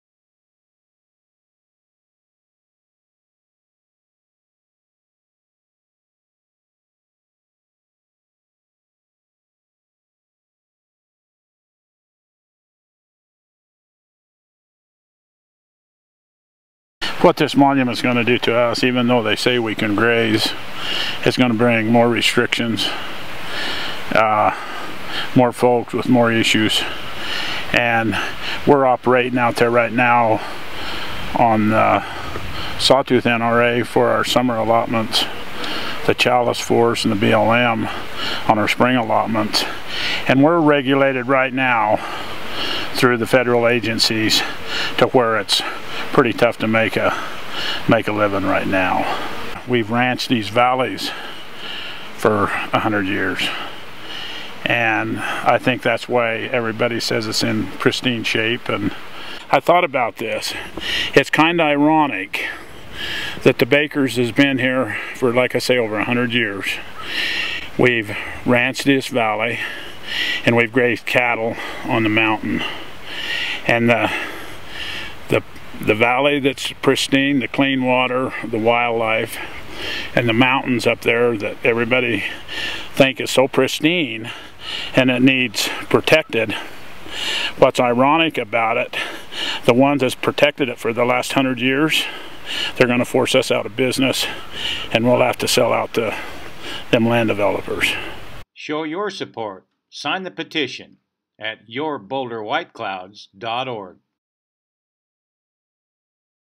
Rancher Doug Baker opposes the National Monument
Rancher Doug Baker opposes the proposed National Monument in the Boulder-White Clouds.